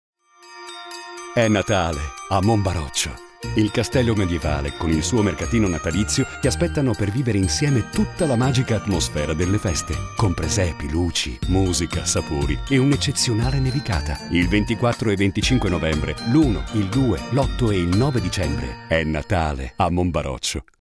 Natale A Mombaroccio 2018 (Official Teaser Full HD)
Gli Appuntamenti di “Natale A Mombaroccio” 2018:

Dall’apertura del Mercatino, fino alle 21:00 un susseguirsi di eventi e animazioni all’interno del borgo medioevale di Mombaroccio.

Mercatino di artigianato artistico, di sapori, di tradizioni, con musica, luci, e colori della tipica atmosfera natalizia.

Eccezionali nevicate si susseguiranno al calar del sole, accompagnate dalle soavi musiche degli zampognari che faranno strada ai 12 Re Magi fino a giungere al Presepe Vivente.

Particolare attenzione ai bambini che potranno immergersi nella magia del Paesaggio Invernale, o nella Casa di Babbo Natale, dove Babbo Natale in persona racconterà fiabe e riceverà le letterine. Lo spazio del Teatro Comunale sarà interamente dedicato ai più piccoli, con spettacoli di burattini e fiabe recitate.

“Natale A Mombaroccio” non è soltanto mercatino di Natale, è anche mercatino di Prodotti tipici, dove all’interno del “Chiostro dei Sapori” si potranno degustare alcune delle eccellenze gastronomiche del nostro territorio, e poi ancora arte e cultura, durante tutta la manifestazione sarà possibile visitare il Museo della Civiltà Contadina (museo che ha partecipato alla creazione dell’ambientazione storica del film “Il Giovane Favoloso” di Martone), dell’Arte Sacra e del Ricamo.

Inoltre vi ricordiamo che siamo presenti su Facebook, Instagram e Twitter con i profili ufficiali Natale A Mombaroccio e Pro Loco Mombaroccio.

Vi aspettiamo per questa edizione e vi ricordiamo le date:

24/25 novembre 2018 e 1/2/8/9 dicembre 2018

Non mancate!